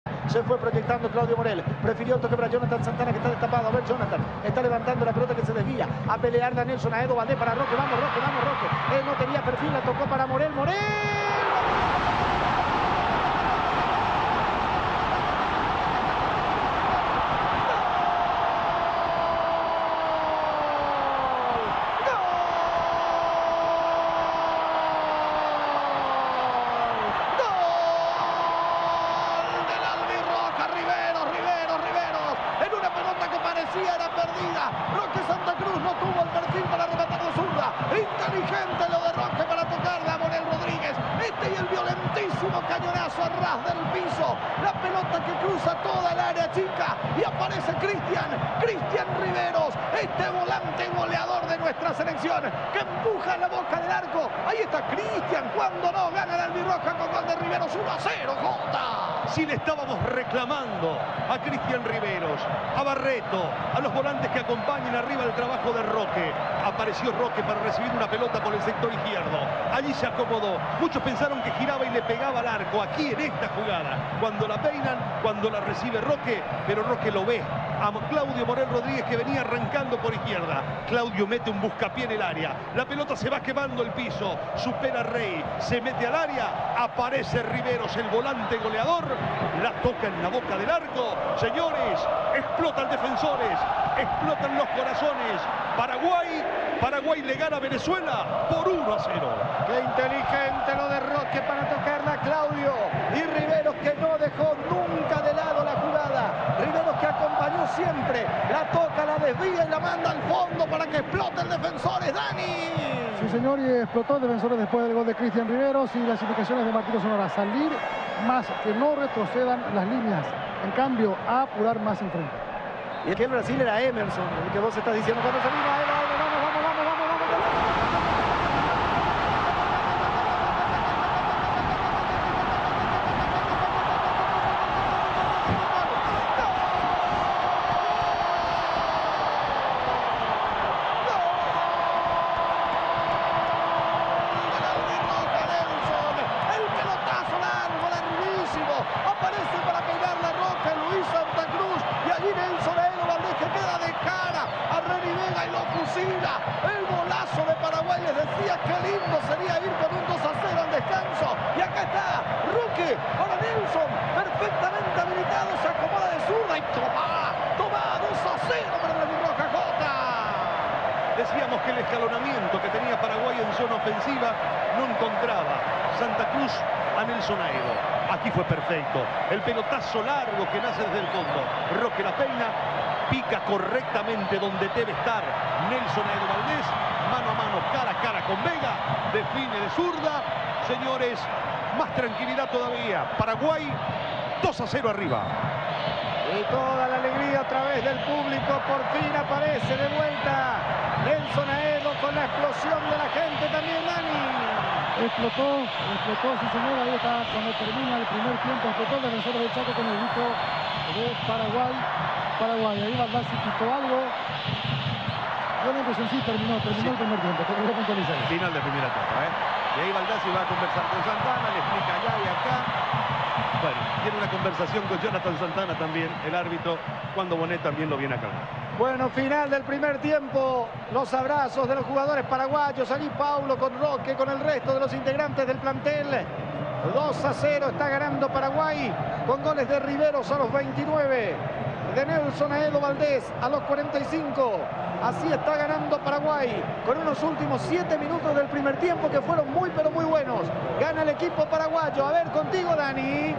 Paraguay 2 vs. Venezuela 0 Eliminatorias Sudáfrica 2010 Salvador Hicar
Paraguay gana y es más puntero que nunca

Paraguay, sin mucho esfuerzo, derrotó 2 a 0 a Venezuela y se consolida como líder de las Eliminatorias Sudamericanas rumbo al Mundial Sudáfrica 2010. La albirroja suma 17 puntos y comanda la tabla sobre 8 fechas disputadas. Christian Riveros y Nelson Haedo Valdez, marcaron los goles albirrojos, ambos en el primer tiempo. La segunda mitad fue casi un trámite.

Relatos: Salvador Hicar
Comentarios: J. J. Bernabé

Grabación y edición: Jorge L. Riquelme